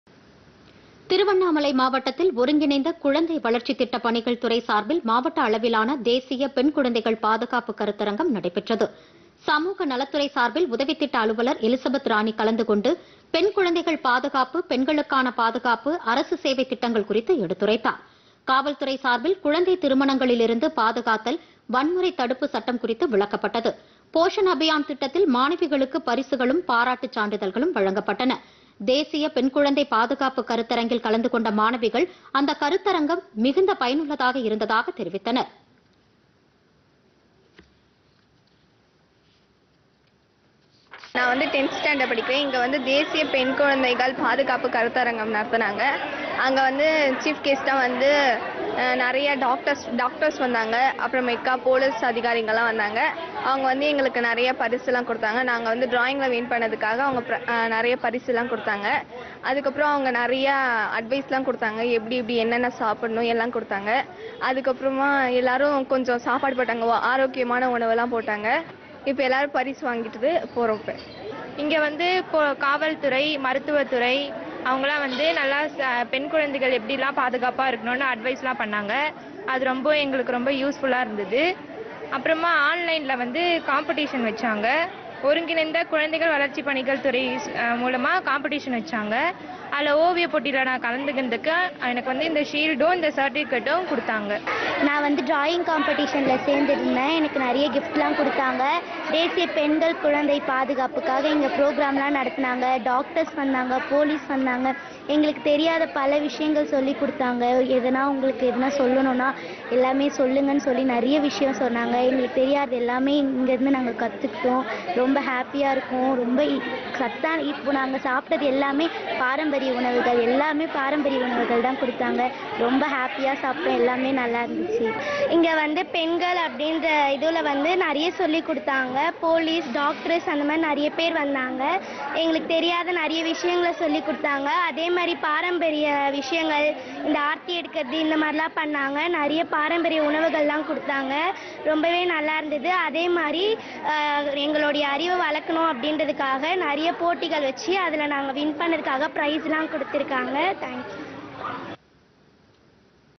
PodhigaiTamilNews பொதிகைசெய்திகள்
திருவண்ணாமலை மாவட்டத்தில் ஒருங்கிணைந்த குழந்தை வளர்ச்சி திட்ட
பணிகள் துறை சார்பில் மாவட்ட அளவிலான  தேசிய  பெண் குழந்தைகள்
பாதுகாப்பு கருத்தரங்கம் நடைபெற்றது.Visit : TamilNews - Doordarshan